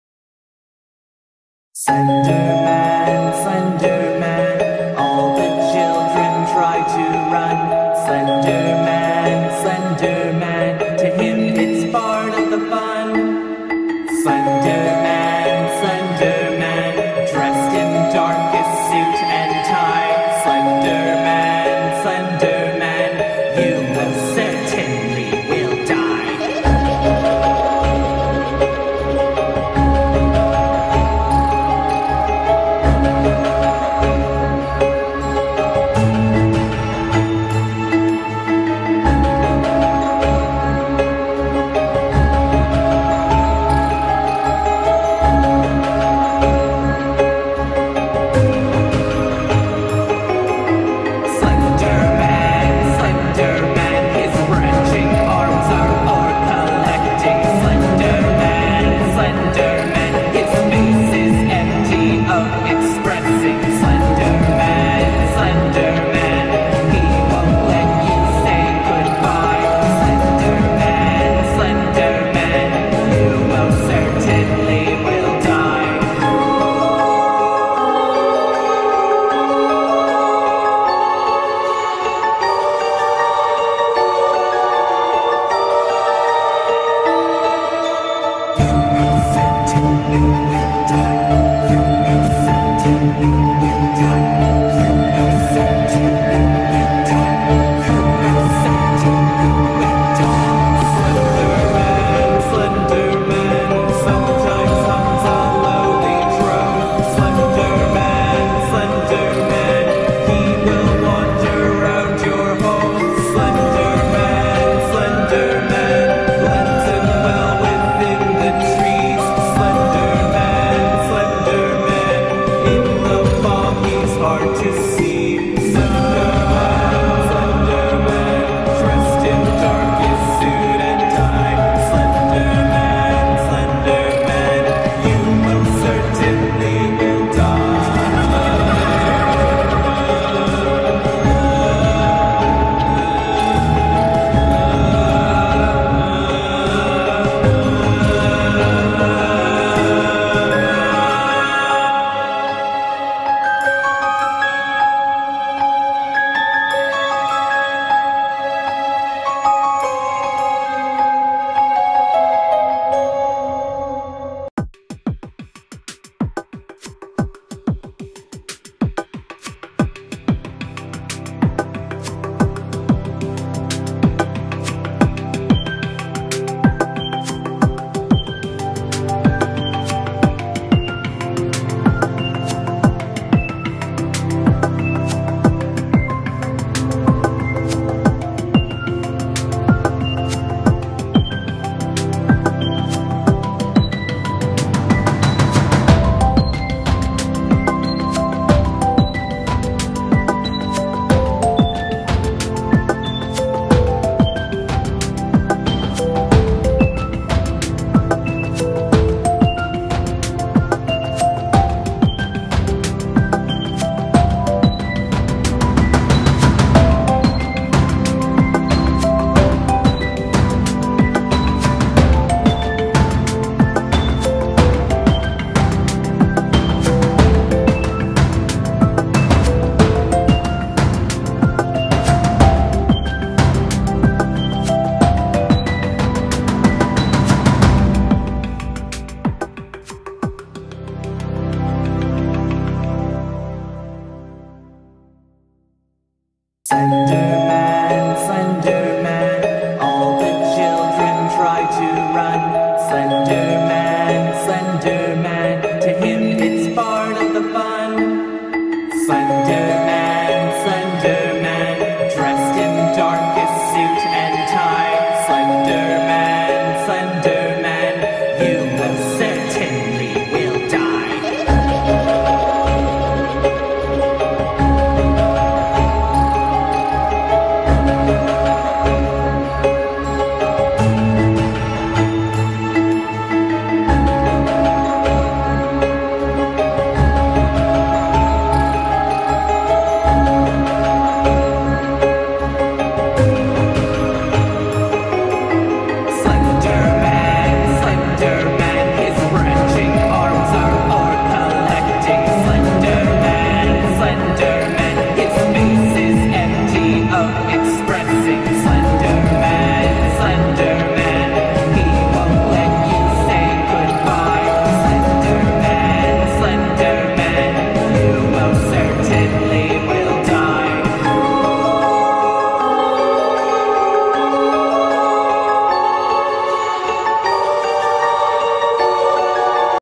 Daycare crew as creepypastas - Slendercornn 3/6 (slenderman) ~speedpaint~
I had fun making this one 😝